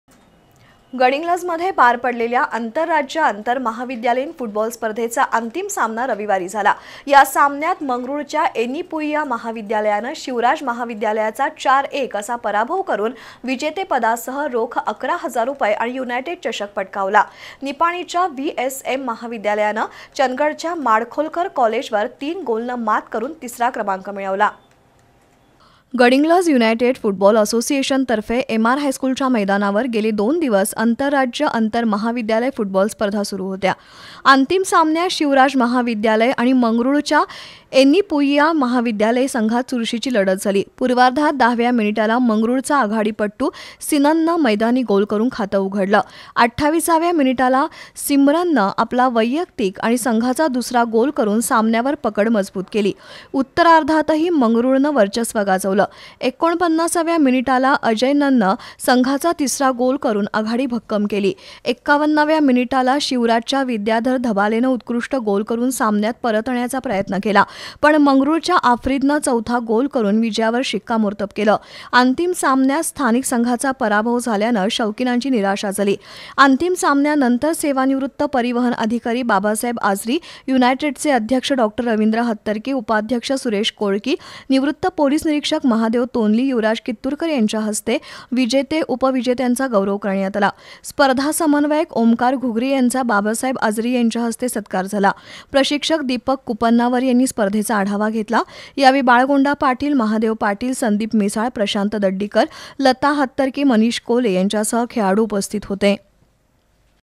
आंतरराज्य आंतरमहाविद्यालयीन फुटबॉल स्पर्धेत मंगळूरच्या येनिपुईया महाविद्यालयाला विजेतेपद
गडहिंग्लजमधील आंतरराज्य आंतरमहाविद्यालयीन फुटबॉल स्पर्धेत मंगळूरच्या येनिपुईया महाविद्यालयाला विजेतेपद